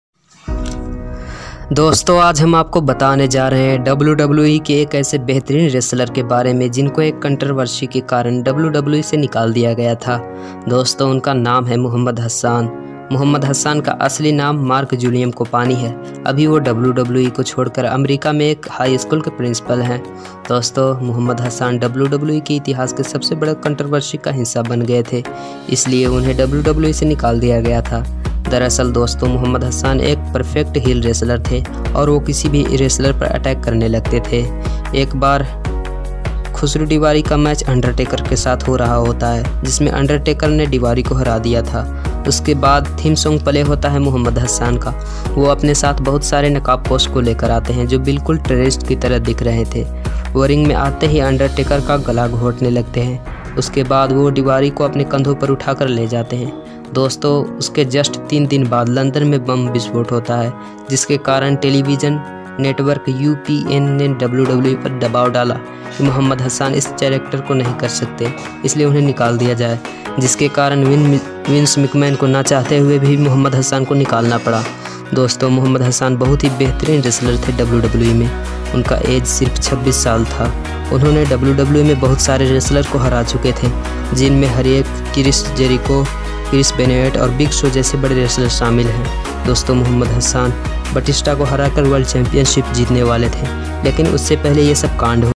Why was Mohammad Hassan fired from WWE. Terrorist Attack WWE.
Why was Mohammad Hassan fired from WWE? Wrestling story of Mohammad Hassan. (wrestling network)

storey,
Once Mohammad Hassan brought terrorists with him to WWE and attacked Undertaker and strangled him.That terrorist attack is followed by a bomb blast in London, which Mohammed Hassan is accused of and he is fired from WWE.

Undertaker vs Mohammed Hasan
Mohammed Hasan terrorist attack on Undertaker. wrestling fact